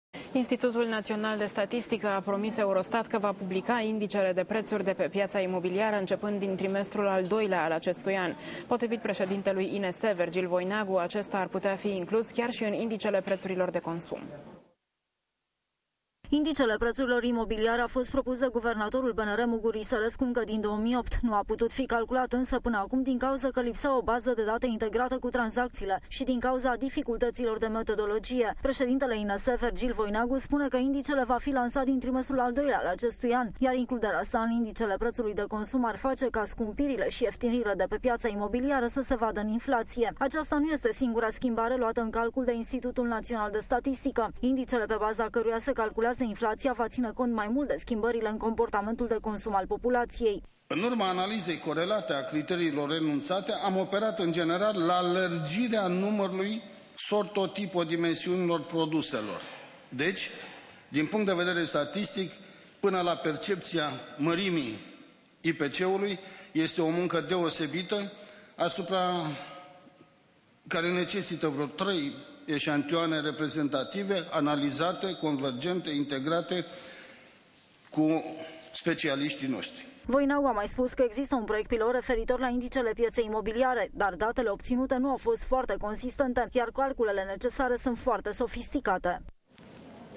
Indicele imobiliar
INS a promis Eurostat ca va publica indicele de preturi de pe piata imobiliara incepand din trimestrul al doilea.